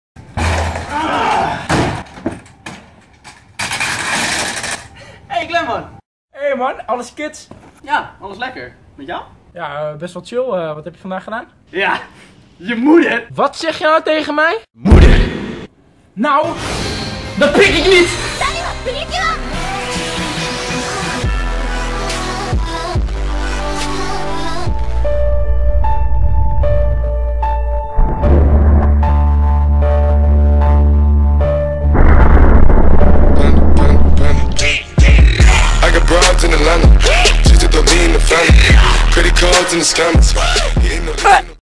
Ruzie Met Mijn Beste Vriend - (GAAT FOUT)
Mijn "Vriend" maakte laatst een abnormaale grap, dit pikte ik niet omdat het over mijn moeder ging, en niemand praat zo over mijn mama.
Er was nog maar een ding te doen, en dat was hem een goeie mep verkopen, nahja hij kreeg hem gratis en voor niks... en op zijn smoel
gelukkig stond mijn ander vriend papblo escabar alles te filmen.
Shoutout to mijn boyz, snapking armoowasright en dutchtuber en RedelijkRanzig natuurlijk :)

tegenwoordig doet iedereen aan clickbait, dus ik dacht waarom ook niet.

Geniet,

xx Papa Glen